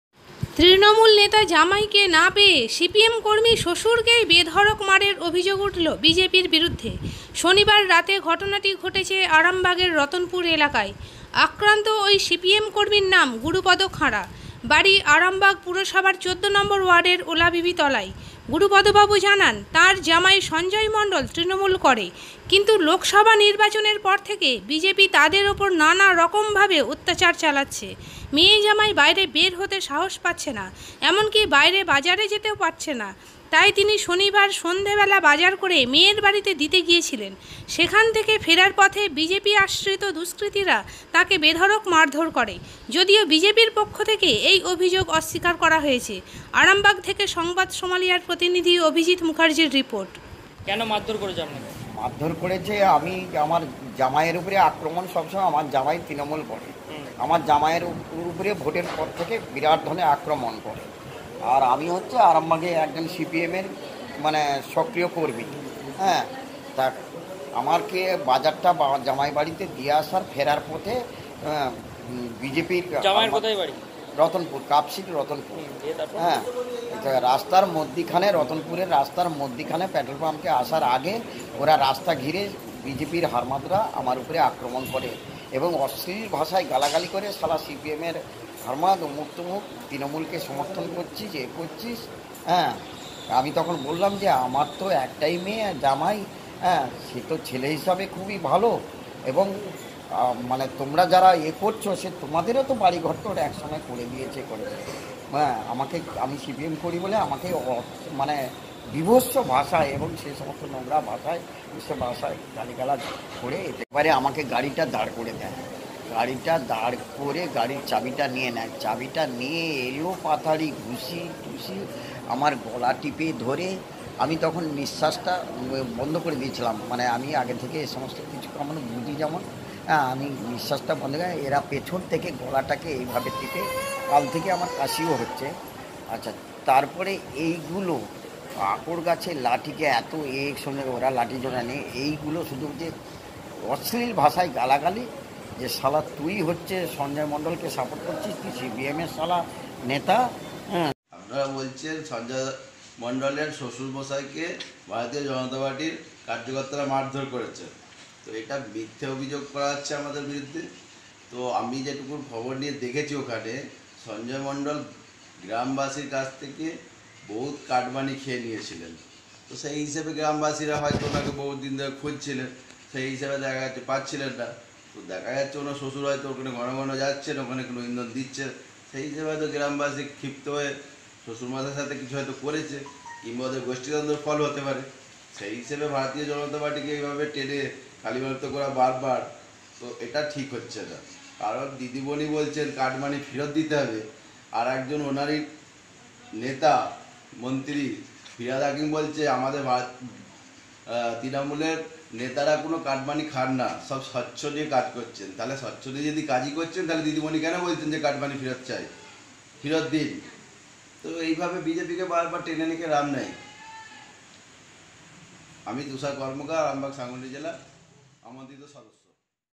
জামাই তৃণমূল নেতা, তাই সিপিএম কর্মী শ্বশুরকে মারধর, অভিযুক্ত বিজেপি
Sambad Somalia @ জামাই তৃণমূল নেতা, তাই সিপিএম কর্মী
শ্বশুরকে মারধর, অভিযুক্ত বিজেপি